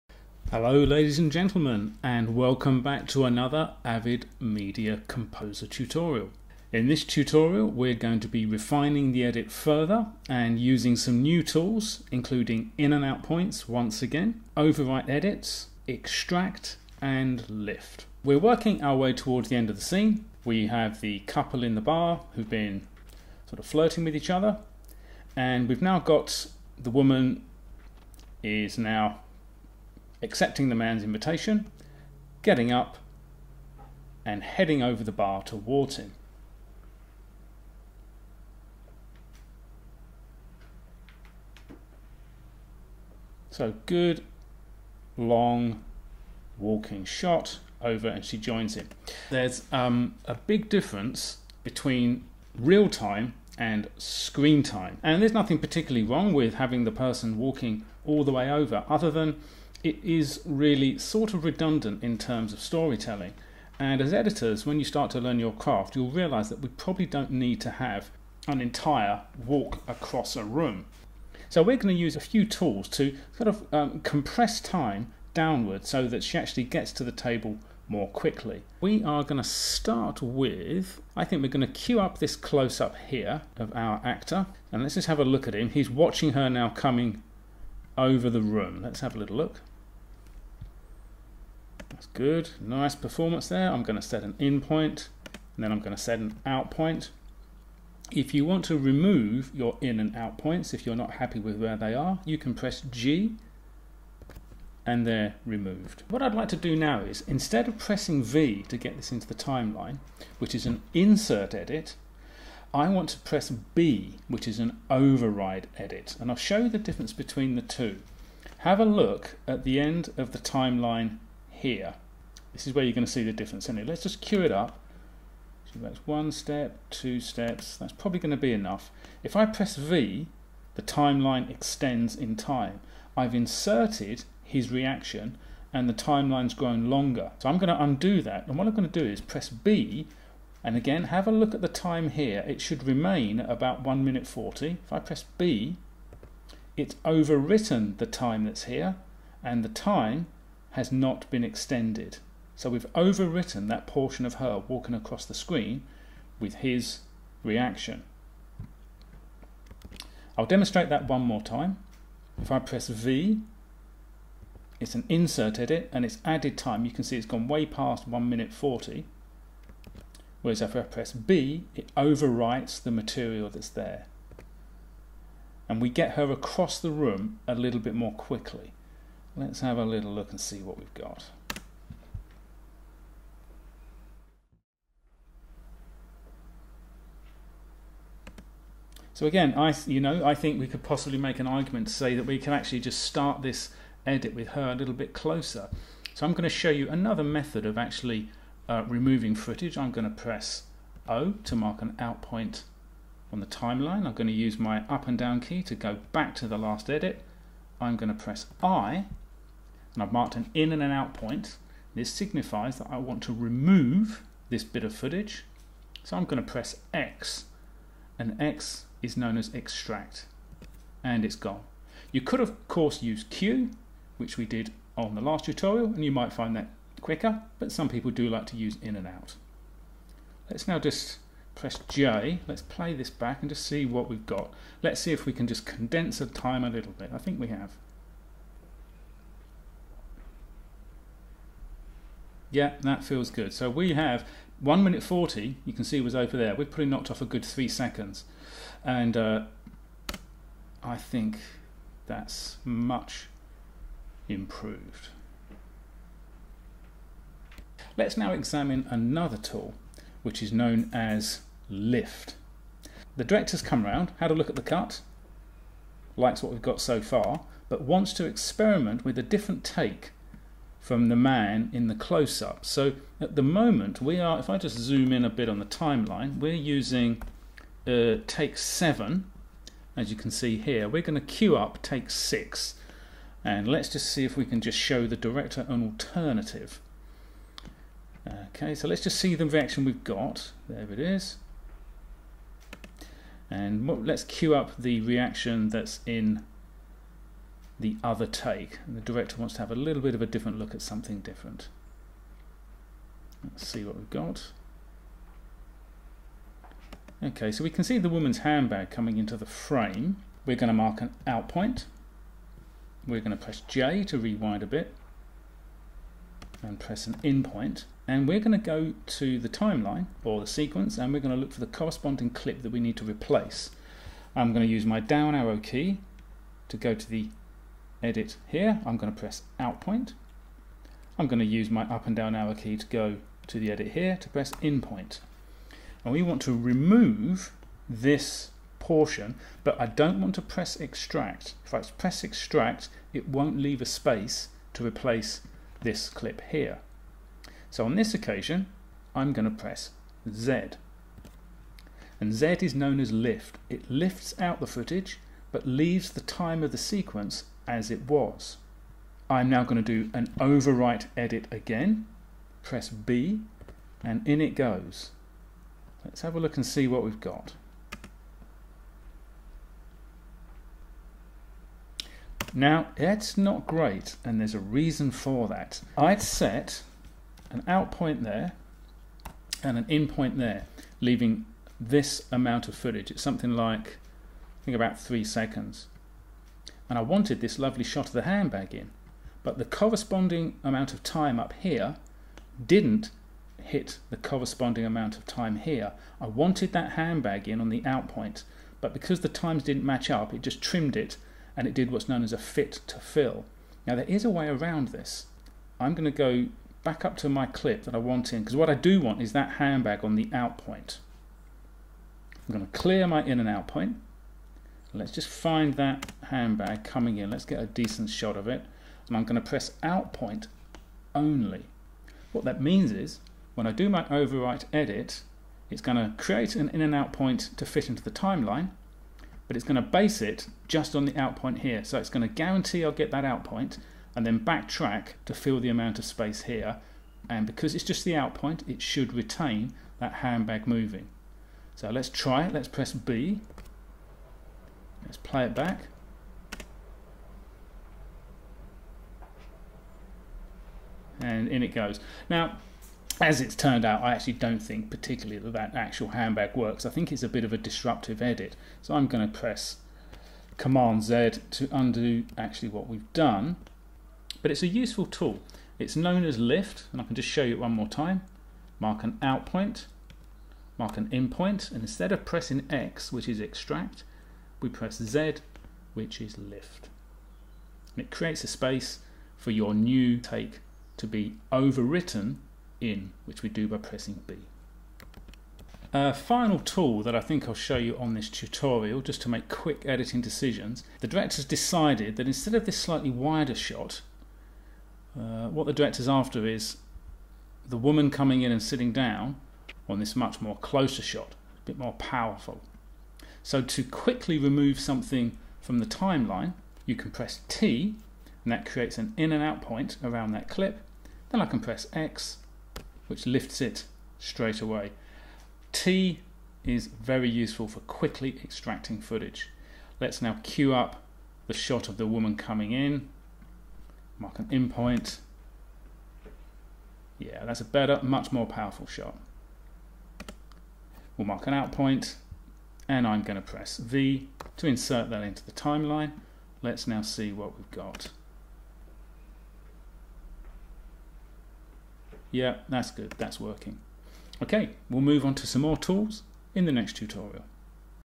Learn Avid Media Composer Fast - Tutorial 4 Overwrite, Extract and Lift Editing
How to refine your edit further using the Overwrite, Lift and Extract Tools, plus a quick look at 3-point editing.